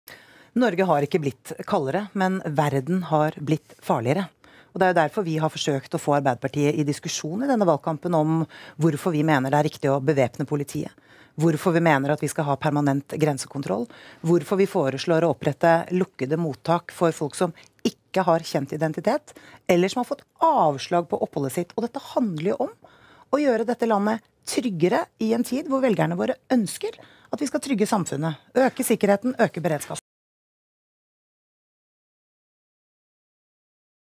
Norge er ikke blitt kaldere, men verden har blitt farligere - Siv Jensen
Siv Jensen møtte Jonas Gahr Støre til debatt på VGTV 6. september 2017.